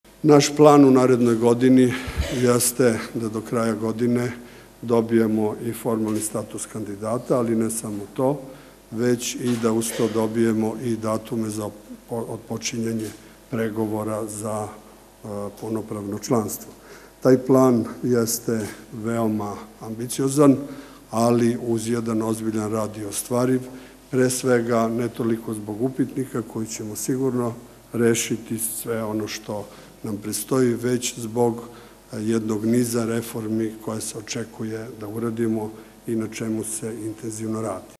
Ekonomska politika bazirana na evrointegracijama (1/3)
(Beograd, 17. decembra 2010) Dvanaesta sednica Skupštine Privredne komore Srbije kojoj je prisustvovao i premijer Srbije Mirko Cvetković. (za više informacija posetite